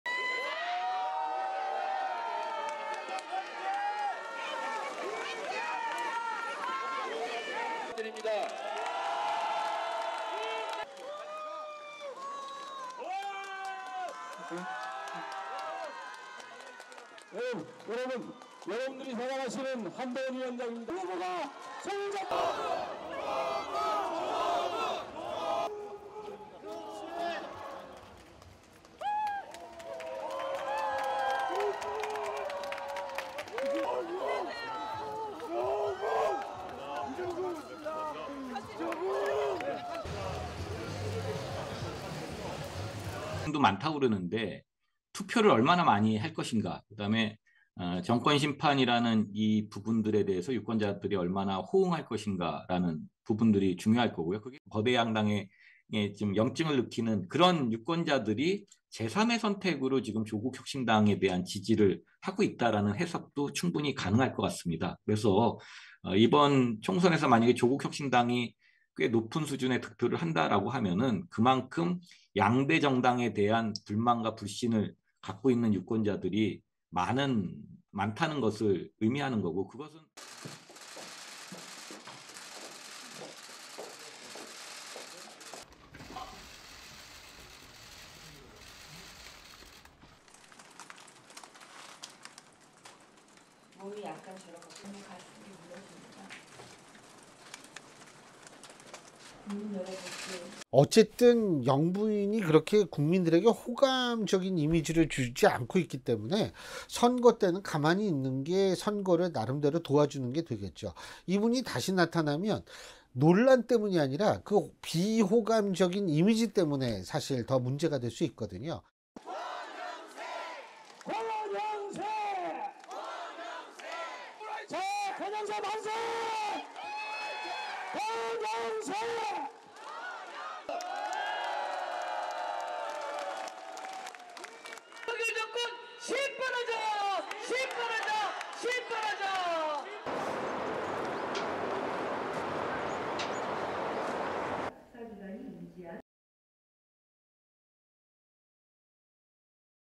韩国将举行国会选举 “祖国革新党”可能成为关键第三势力
韩国周三（4月10日）即將舉行国会选举，目前国会300席中，由反对党共同民主党掌握绝对多数席次，执政党国民力量党盼翻转朝小野大局面，但面临艰难斗争。德成女子大学政治学和国际研究教授曹镇万（CHO JIN-MAN）表示，前法务部长曹国（CHO KUK）创建的“祖国革新党”，可能成为两大政党在选举中努力获得多数席次的潜在破坏者。